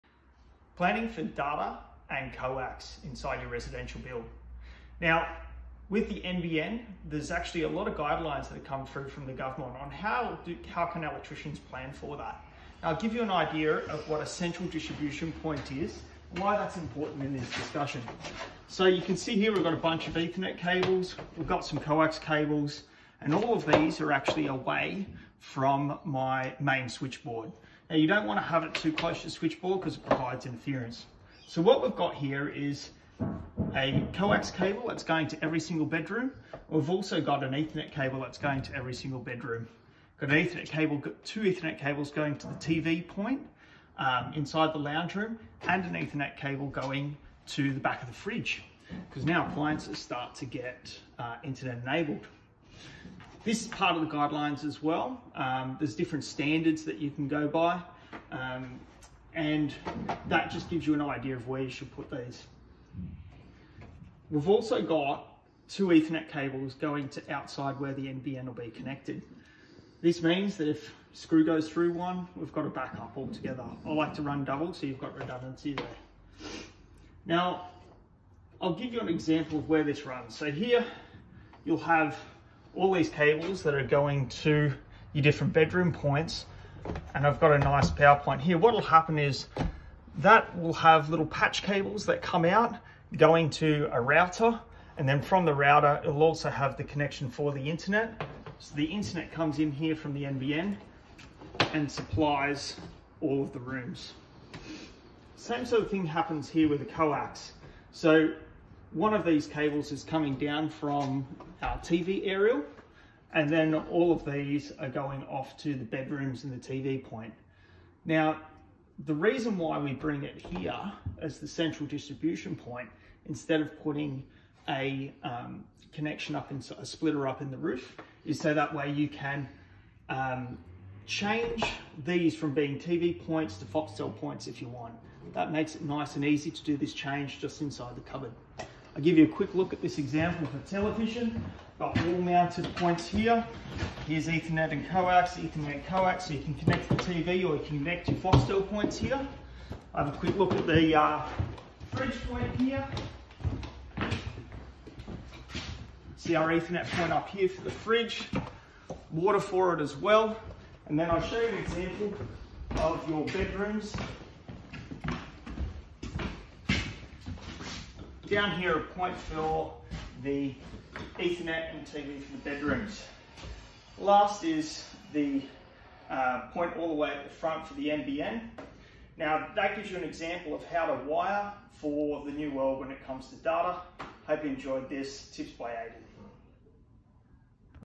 In home cabling for data and tv ready for high speed nbn and internet enabled devices
A tour of In home cabling for data and tv ready for high speed nbn and internet enabled devices.

We cover:
Location of central distribution point(cdp) where you put your router and distance from electrical panel
Ethernet from exterior connection to cdp
Ethernet for bedrooms, kitchen, tv
Coax for bedrooms, Lounge and aerial and why coax should be at the cdp

Ref:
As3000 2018

A licences electrician is required for installation.